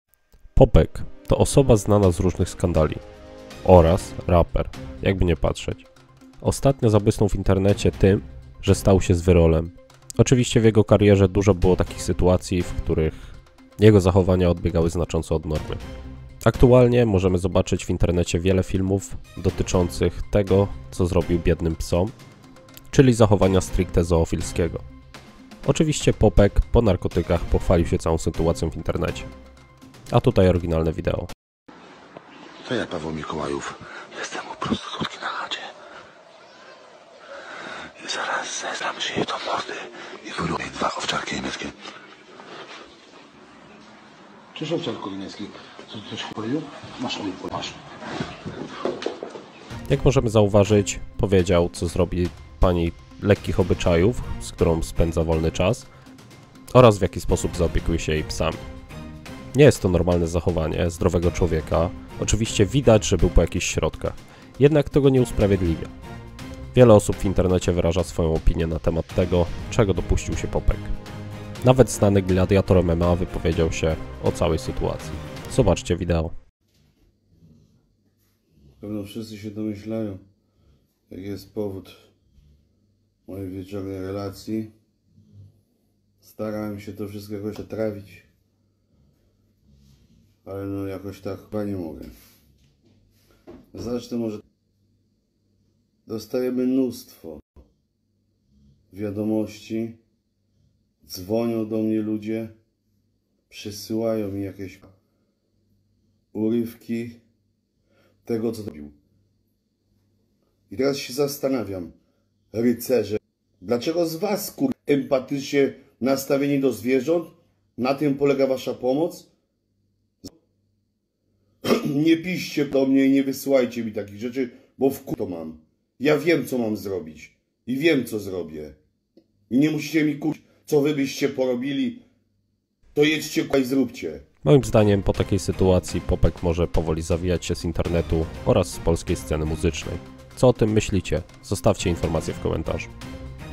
Krótki NEWS ⚠️ - Upadek Popka
Po kontrowersyjnym nagraniu raper Popek zostaje wciągnięty w wir publicznej burzy po skandalicznym zachowaniu wobec psów. Jego dotychczasowi fani odwracają się od niego, a społeczność internetowa domaga się konsekwencji. Film ukazuje dramatyczne wydarzenia, które doprowadziły do załamania kariery artysty, a także próby odbudowania swojego wizerunku w obliczu społecznej krytyki. Czy Popek będzie w stanie zmienić swoje postępowanie i odzyskać zaufanie swoich fanów, czy też jego kariera przyniesie mu konsekwencje za skandal?

Videos by from Pixels